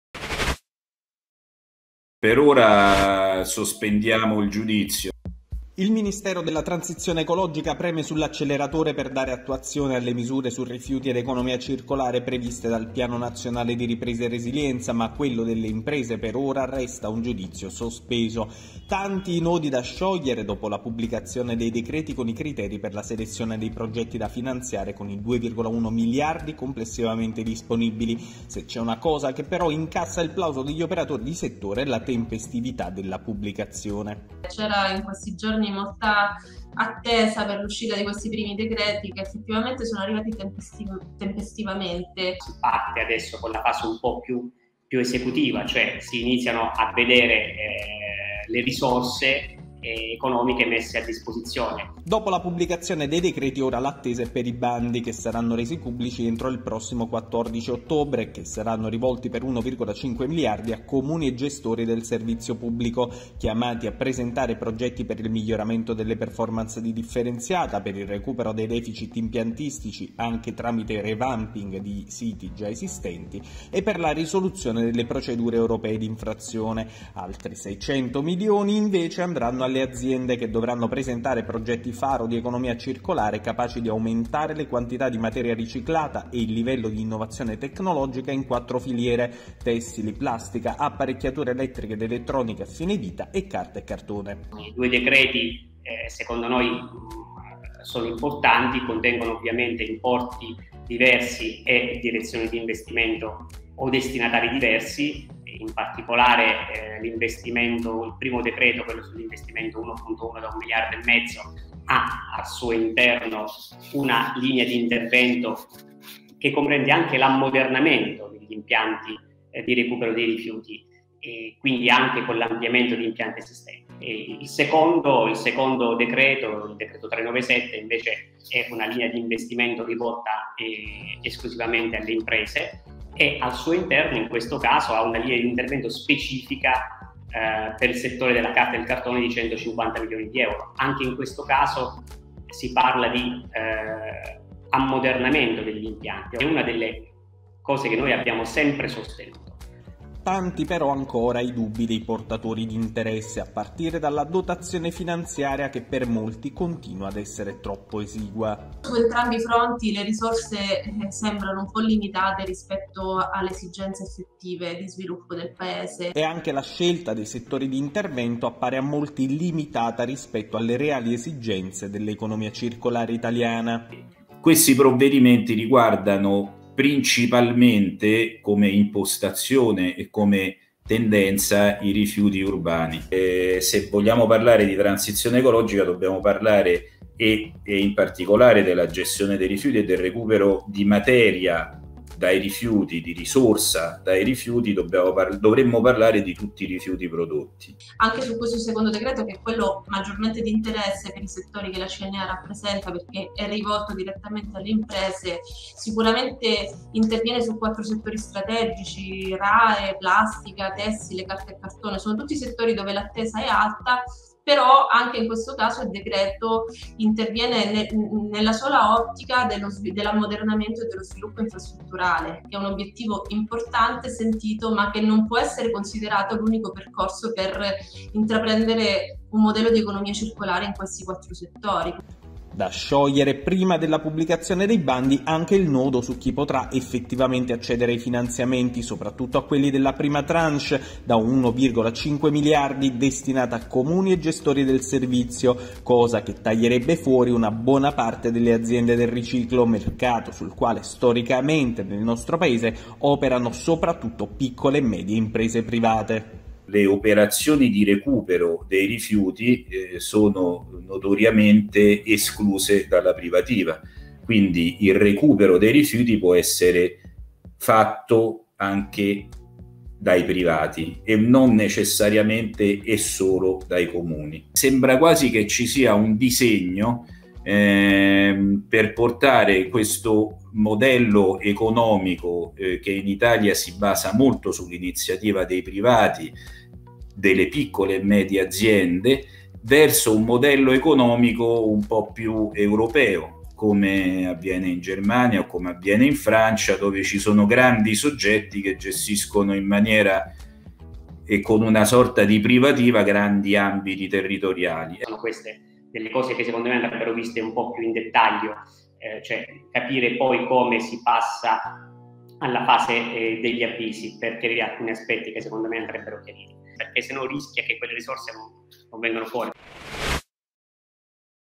I timori delle imprese sui decreti MITE
Pochi fondi, ambito d'applicazione limitato e troppi ostacoli al finanziamento per le imprese private del riciclo. I primi dubbi delle associazioni di categoria sui criteri per la selezione dei progetti su rifiuti ed economia circolare da finanziare con i fondi del Pnrr